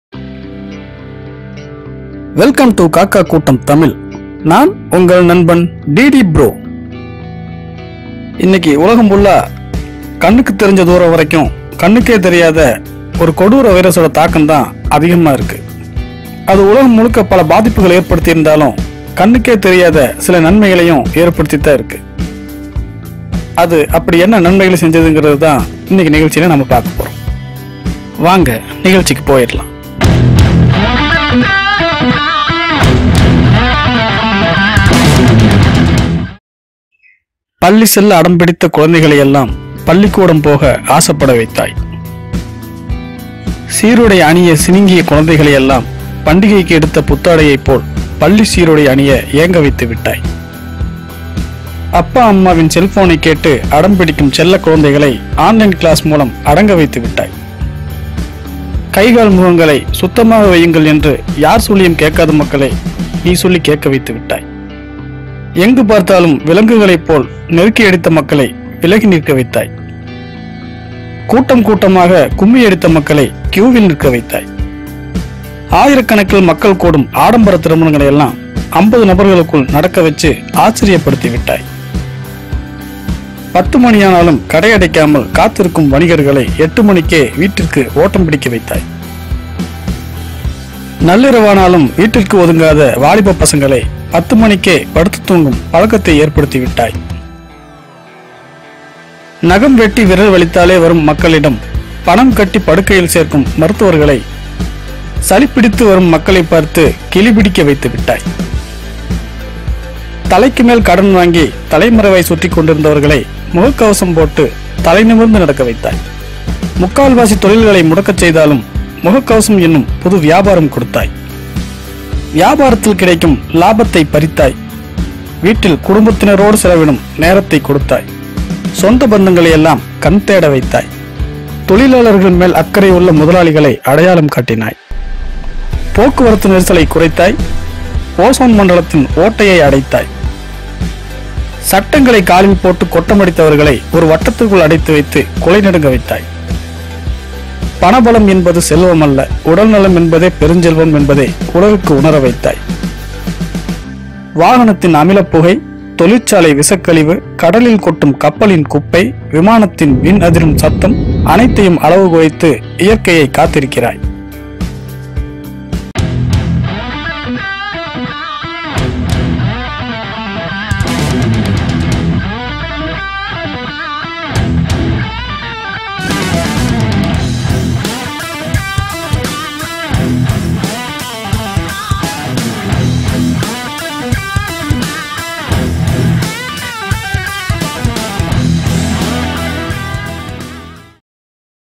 லாக் டவுன் நன்மைகள் | Kaka Koottam Tamil
This Video Explained Pandemic moments also present opportunity: more sophisticated and flexible use of technology, less polarization, a revived appreciation for the outdoors and life’s other simple pleasures. No one knows exactly what will come, but here is our best stab at a guide to the unknown ways that society—government, healthcare, the economy, our lifestyles and more—will change.

video & image  credit  to pexals, videezy, pixabay